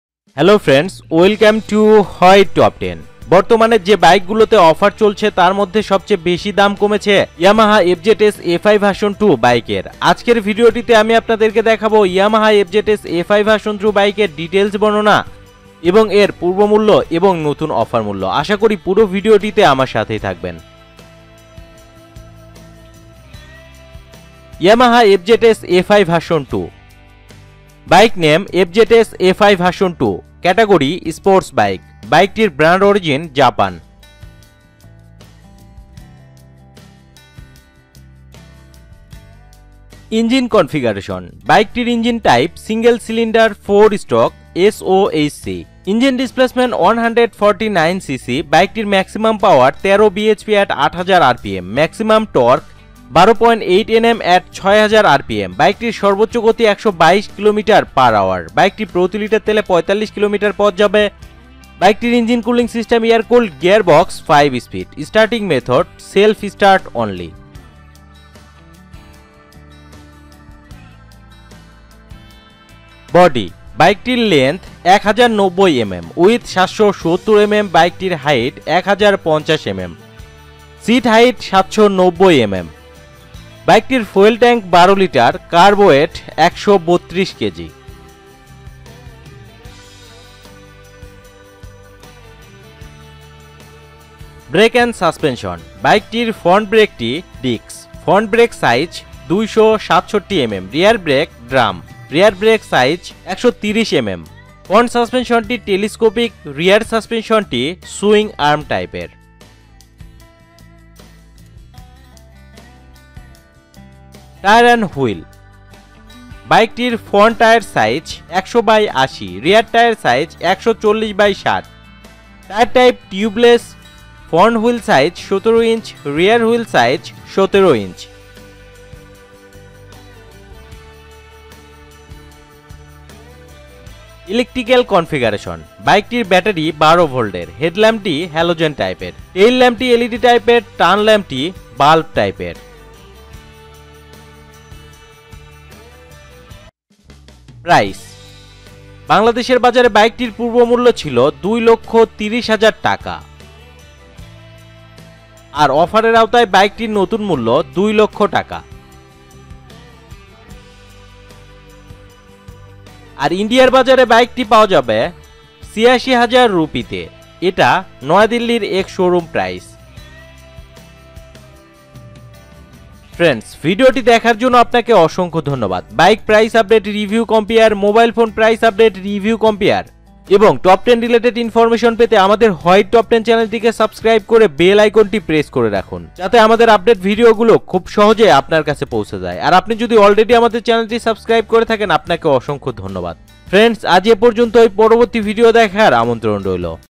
Yamaha FZS FI V2 Big Offer Price in Bangladesh 2020
Yamaha FZS FI V2 Big Offer Price in Bangladesh 2020 i am showing in this Video. Yamaha reduce big price Yamaha FZS FI V2 in Bangladesh. FZS V2 Most Popular Bike in Bangladesh and India. Yamaha FZS FI V2 Offer Price in Bangladesh i am showing in this Video. I am always try to Provide Right information although any kind of mistake i am say sorry.